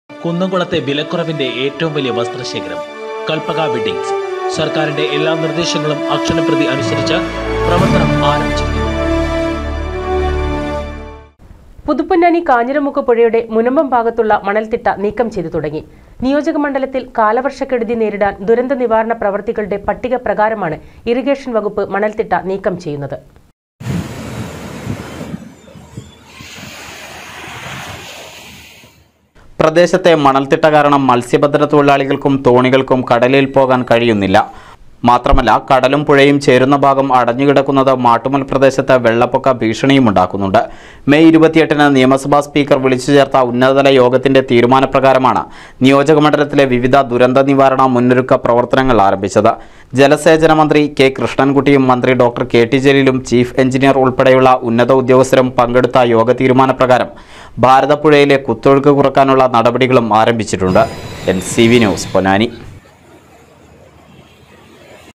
പുതുപൊന്നാനി- കാഞ്ഞിരിമുക്ക് പുഴയുടെ മുനമ്പം ഭാഗത്തുള്ള മണൽതിട്ട നീക്കം ചെയ്തു തുടങ്ങി
പുതുപൊന്നാനി- കാഞ്ഞിരിമുക്ക് പുഴയുടെ മുനമ്പം ഭാഗത്തുള്ള മണൽതിട്ട നീക്കം ചെയ്തു തുടങ്ങി.നിയോജക മണ്ഡലത്തിൽ  കാലവർഷക്കെടുതി നേരിടാൻ ദുരന്ത  നിവാരണ പ്രവൃത്തികളുടെ പട്ടിക പ്രകാരമാണ് ഇറിഗേഷൻ വകുപ്പ് മണൽതിട്ട നീക്കുന്നത്